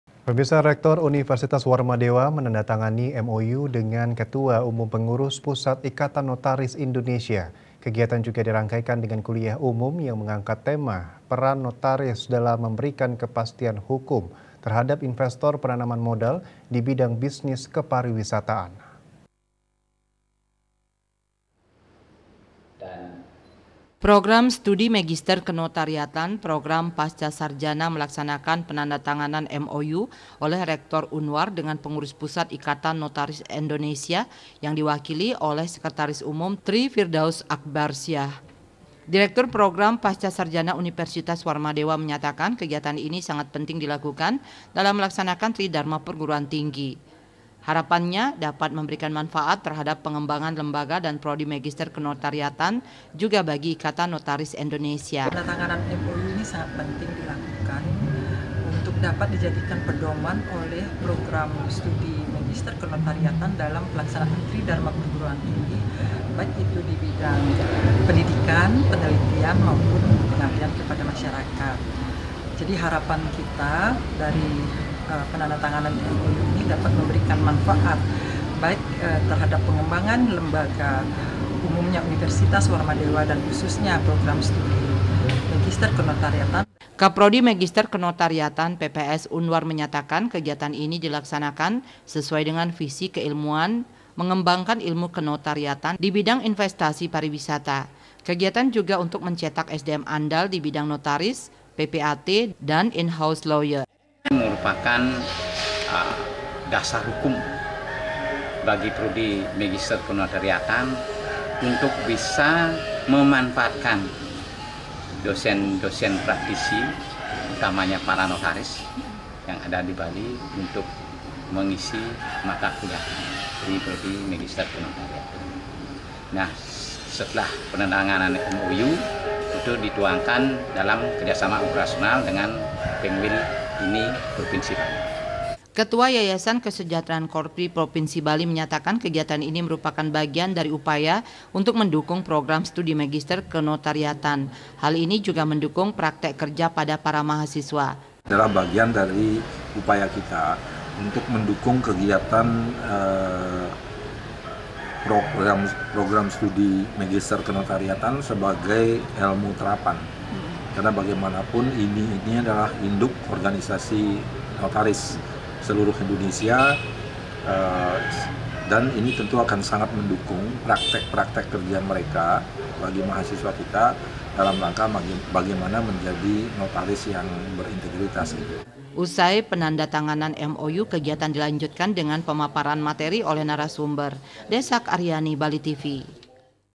PENANDATANGANAN MOU ANTARA REKTOR UNWAR DENGAN KETUA UMUM PENGURUS PUSAT IKATAN NOTARIS INDONESIA
REKTOR UNIVERSITAS WARMADEWA MENANDATANGANI MOU DENGAN KETUA UMUM PENGURUS PUSAT IKATAN NOTARIS INDONESIA. KEGIATAN JUGA DIRANGKAIKAN DENGAN KULIAH UMUM, YANG MENGANGKAT TEMA, “PERAN NOTARIS DALAM MEMBERIKAN KEPASTIAN HUKUM TERHADAP INVESTOR PENANAMAN MODAL, DI BIDANG BISNIS KEPARIWISATAAN”.